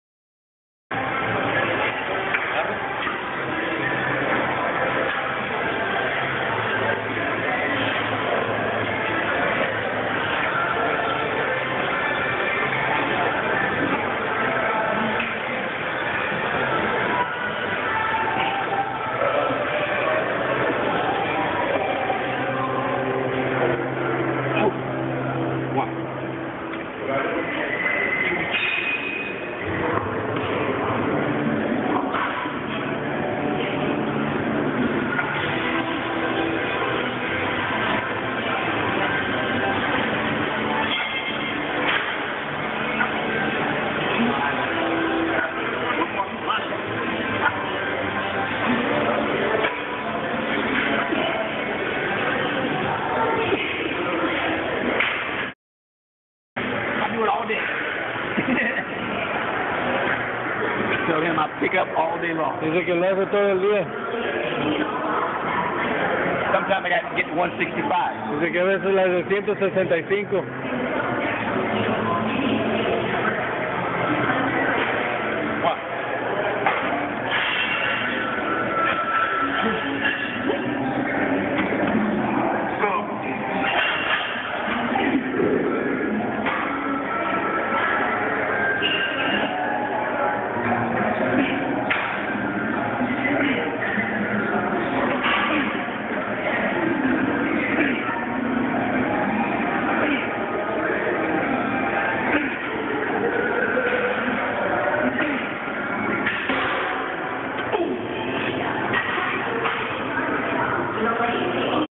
Press inclinada con mancuerna (Charro Lomeli)
IFBB Pro Charro Lomeli nos da una ensenanza de rutina de pecho superior con press inclinada usando mancuernas muy pesadas!